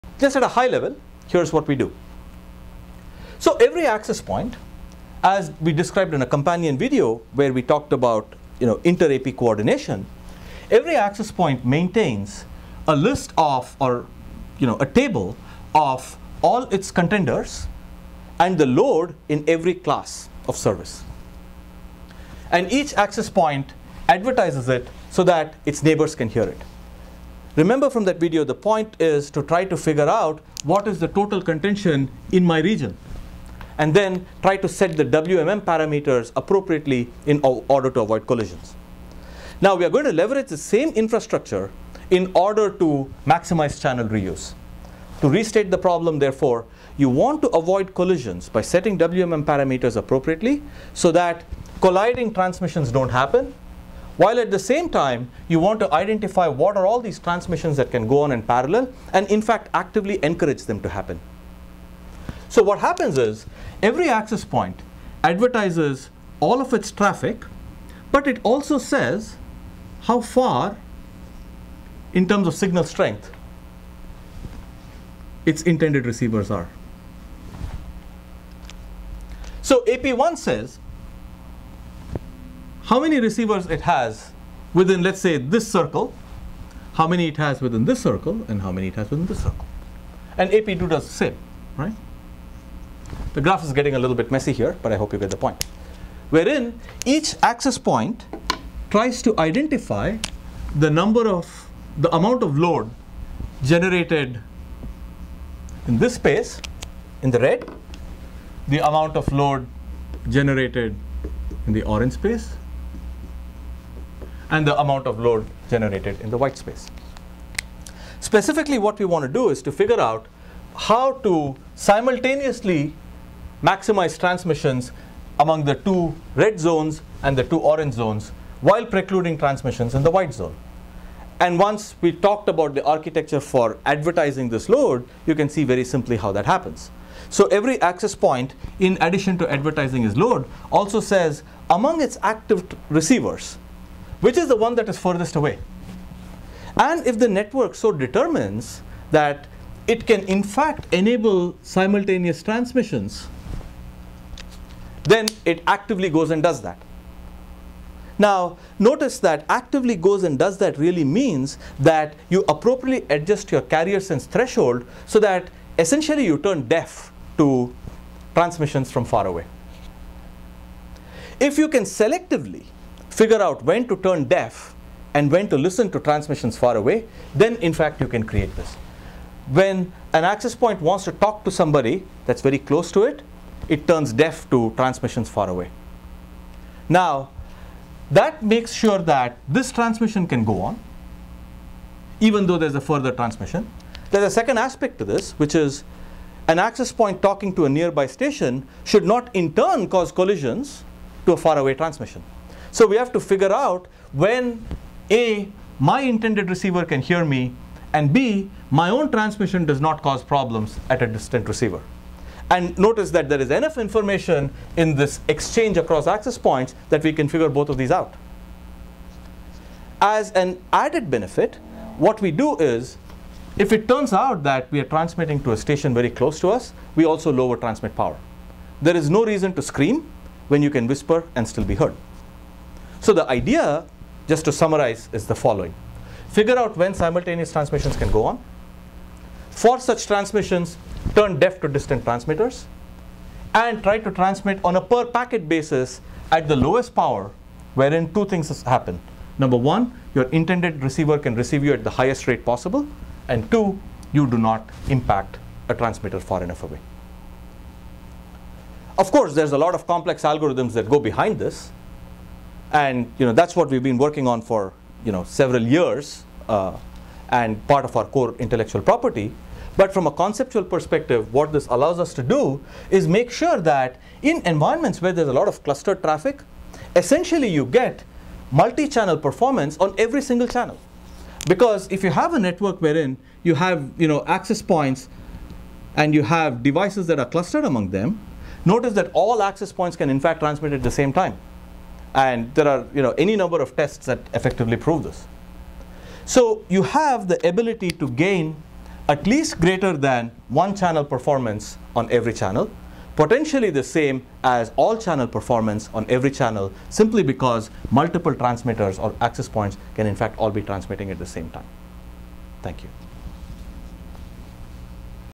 Maximizing Air Traffic - Part 2: Simultaneous Transmissions
Wireless LAN: Maximizing Air Traffic - Part 2 of 2

At a high level, here's what we do. So every access point, as we described in a companion video where we talked about inter-AP coordination, every access point maintains a list or a table of all its contenders and the load in every class of service. And each access point advertises it so that its neighbors can hear it. Remember from that video, the point is to try to figure out what is the total contention in my region, and then try to set the WMN parameters appropriately in order to avoid collisions. Now we are going to leverage the same infrastructure in order to maximize channel reuse.

To restate the problem, therefore, you want to avoid collisions by setting WMN parameters appropriately so that colliding transmissions don't happen, while at the same time you want to identify what are all these transmissions that can go on in parallel and in fact actively encourage them to happen. So what happens is every access point advertises all of its traffic, but it also says how far in terms of signal strength its intended receivers are. So AP1 says how many receivers it has within let's say this circle, how many it has within this circle and how many it has within this circle. And AP2 does the same. The graph is getting a little bit messy here, but I hope you get the point. Within, each access point tries to identify the amount of load generated in this space, in the red, the amount of load generated in the orange space, and the amount of load generated in the white space.

Specifically, what you want to do is figure out how to simultaneously maximize transmissions among the two red zones and the two orange zones while precluding transmission in the white zone, and once we talk about the architecture for advertising this load you can see very simply how that happens. So every access point, in addition to advertising its load, also says among its active receivers, which is the one that is furthest away? And if the network so determines that it can in fact enable simultaneous transmissions, then it actively goes and does that. Now, notice that actively goes and does that really means that you appropriately enter your carrier's threshold so that essentially you turn deaf to transmissions from far away. If you can selectively figure out when to turn deaf and when to listen to transmissions far away, then in fact you can create this. When an access point wants to talk to somebody that's very close to it, it turns deaf to transmissions far away. Now, that makes sure that this transmission can go on even though there's a further transmission.

There's a second aspect to this, which is an access point talking to a nearby station should not in turn cause collisions to a far away transmission. So we have to figure out when A, my intended receiver can hear me, and B, my own transmission does not cause problems with a distant receiver. Notice that there is information in this exchange across access points that we can figure both of these out. As an added benefit, what we do is if it turns out that we are transmitting to a station very close to us, we also lower transmit power. There is no reason to scream when you can whisper and still be heard.

So the idea, just to summarize, is the following. Figure out when simultaneous transmissions can go on, force such transmissions, turn deaf to distant transmitters, and try to transmit on a per-packet basis at the lowest power wherein two things happen. Number one, your intended receiver can receive at the highest rate possible, and two, you do not impact a transmitter far enough away. Of course, there's a lot of complex algorithms that go behind this and that's what we've been working on for several years, and part of our core intellectual property, but from a conceptual perspective what this allows us to do is make sure that in environments where there's a lot of clustered traffic, essentially you get multi-channel performance on every single channel, because if you have a network wherein you have access points and you have devices that are clustered among them, notice that all access points can in fact transmit at the same time, and there are any number of tests that effectively prove this. So you have the ability to gain at least greater than one channel performance on every channel, potentially the same as all-channel performance on every channel simply because multiple transmitters or access points can in fact all be transmitting at the same time. Thank you.